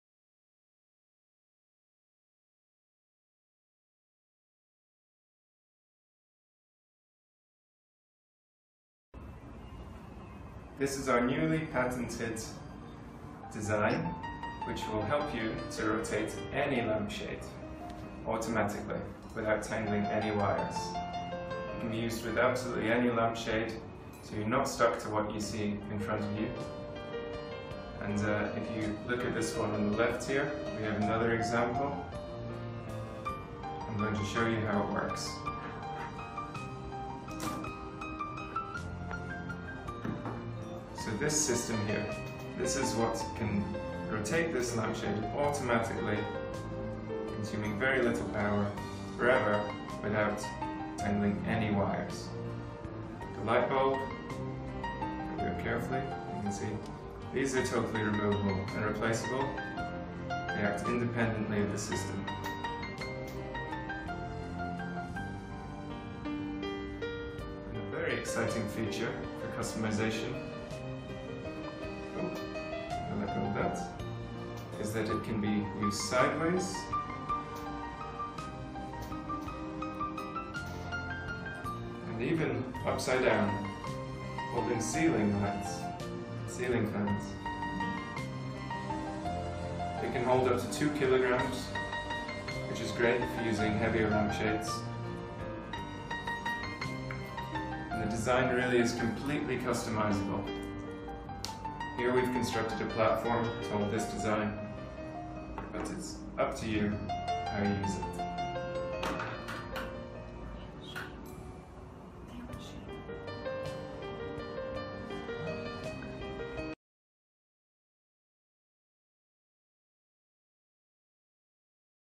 automatic rotating light
any lampshades can rotate automaticallly
it can be used for table lamp, floor lamp, ceiling lamp and wall lights.
lampshade can be customzied according to different concepts and designs.
The lighting head can hold 2kgs lampshades.